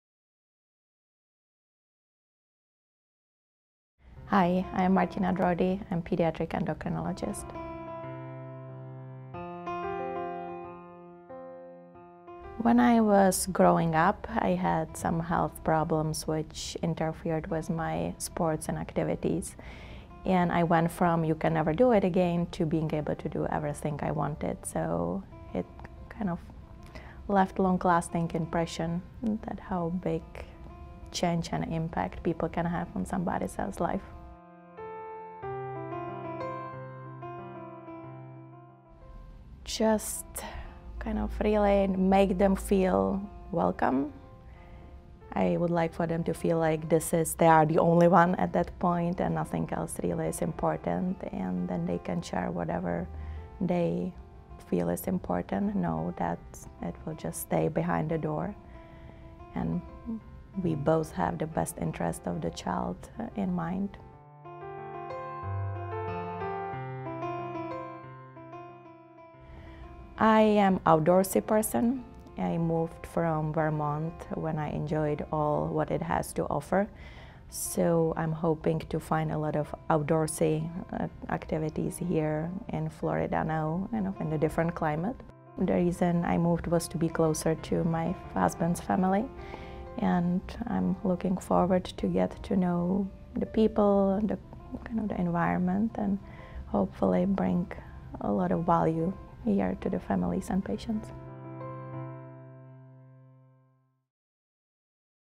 Meet Martina Drawdy, MD, Pediatric Endocrinology | Ascension Florida
Meet Martina Drawdy, MD, a Pediatric Endocrinology provider with Ascension in Pensacola, FL. More conversation. More questions and answers. Ascension's care team works together to make sure you're getting the best care possible.